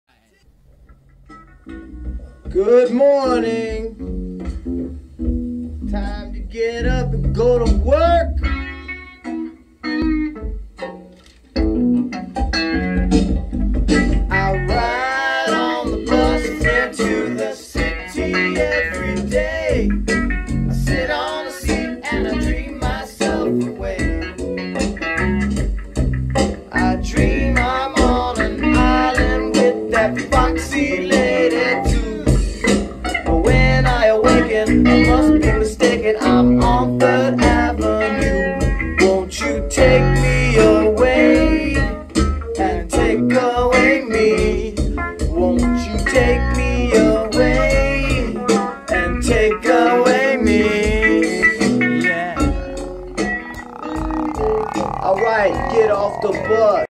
Morning Commute - The downhill...........
Starting at home, another Richmond to Richmond commute today.  Get off the Bus!